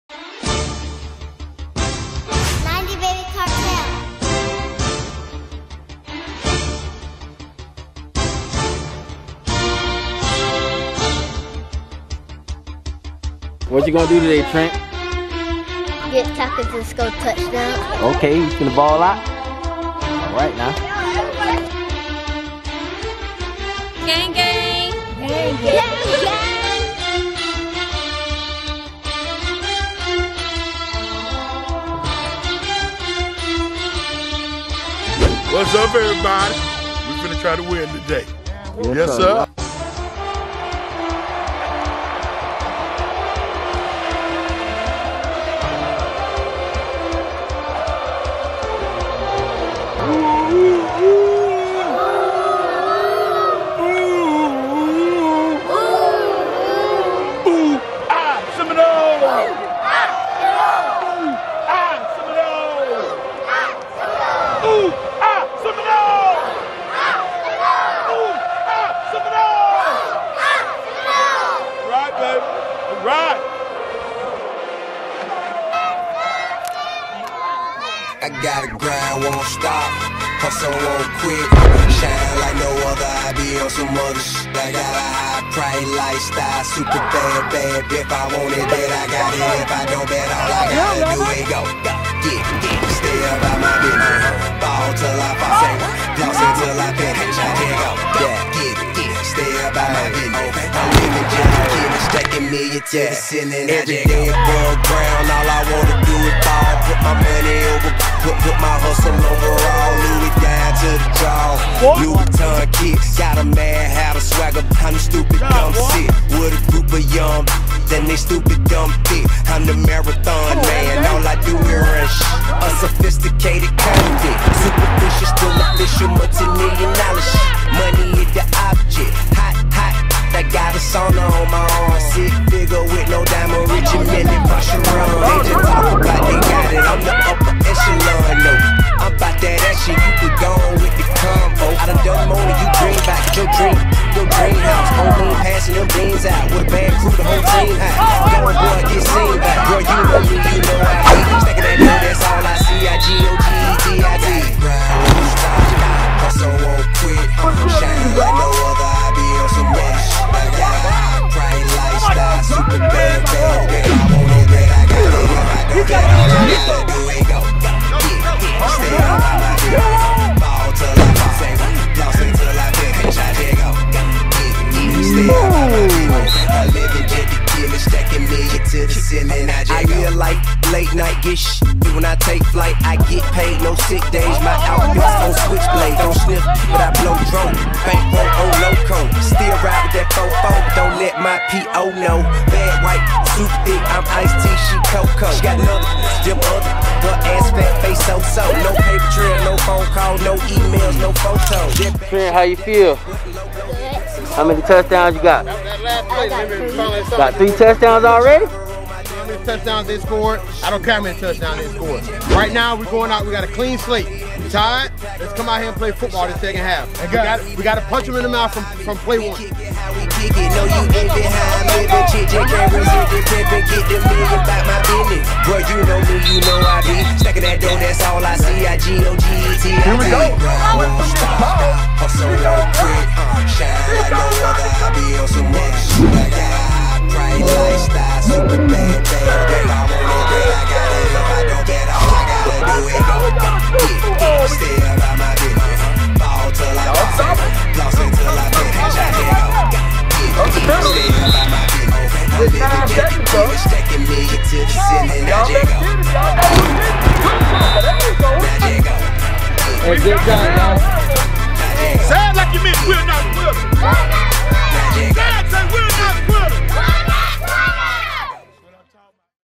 90BC Presents - Sanford Seminole 6u vs Pine Hills Trojans
Hard fought battle for Seminole 6u vs the Trojans and they lost by 2pts. They fought to the very end and chanted, WE ARE NOT QUITERS! It was a great day for football and they fired touchdown after touchdown at one another until the Trojans took over in the 4th quarter. This one is going down in the books! Great game and great sportsmanship all around!